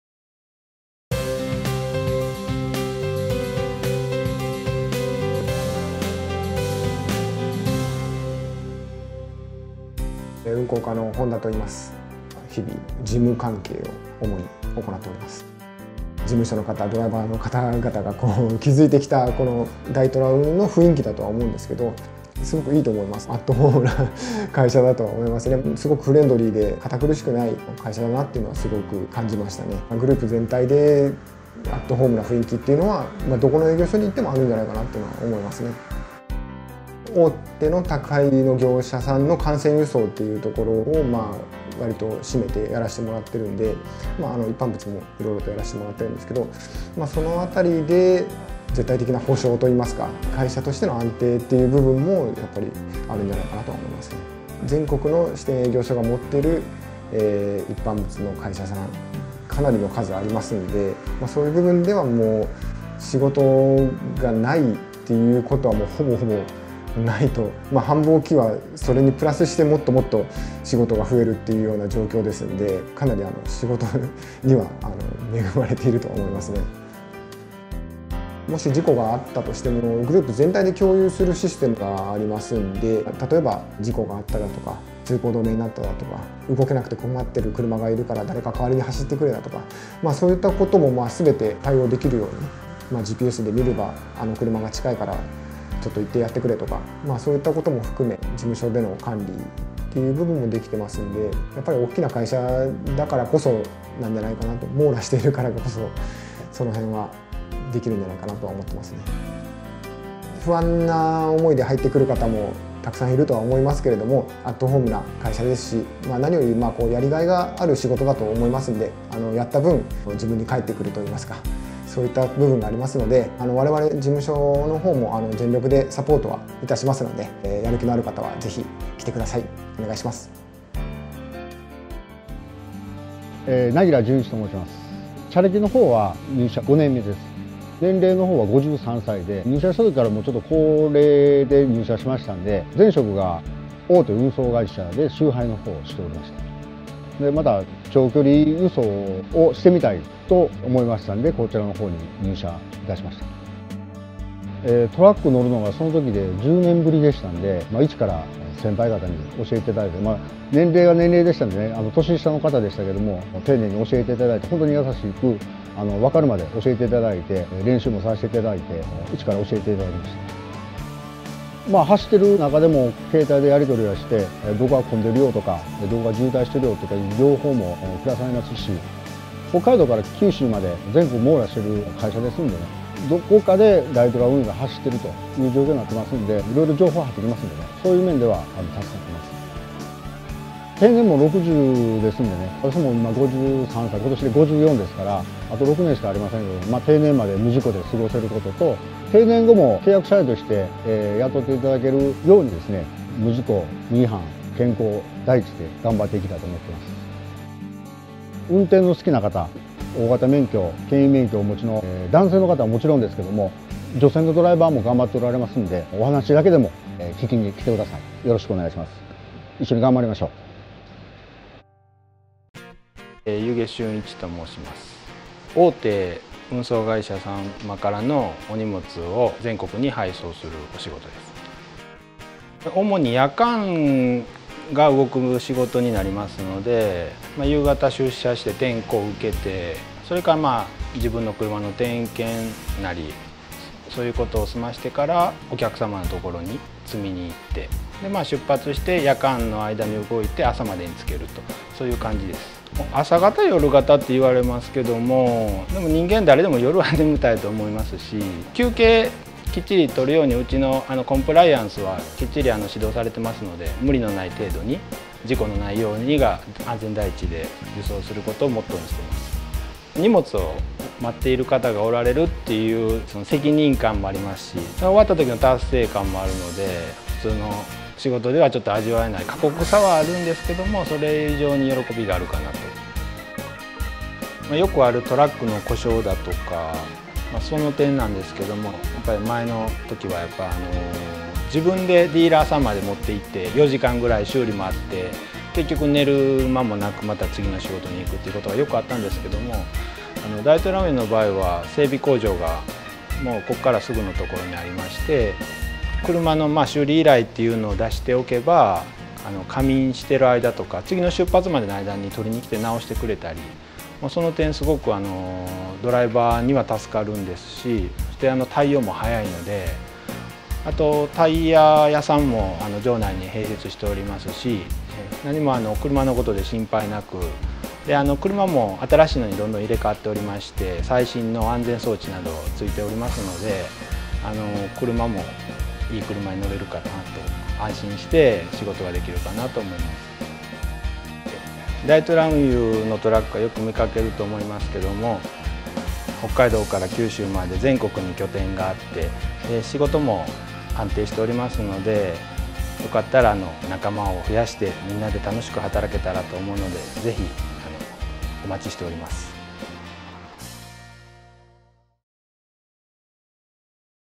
ドライバー求人 ドラEVER【大虎運輸グループ】
【大虎運輸グループ】
ドライバーの求人情報に特化した求人サイト「ドラEVER」です。
新着企業、注目、おすすめなどを、いち早く閲覧できるバナーを設定。自分の希望を細かく絞り込める検索機能などが充実しています。
また他のドライバー求人サイトと大きく違う点は、なんといっても企業のインタビュー動画が掲載されていること。文字情報だけでは伝わらない、会社の雰囲気や実際に働いている人たちの人柄をインタビュー動画から感じ取れるのが「ドラEVER」最大の特徴です。

また掲載をご検討される企業様も是非一度「ドラEVER」をご覧いただき、当サイトの素晴らしさをご確認頂ければと思います。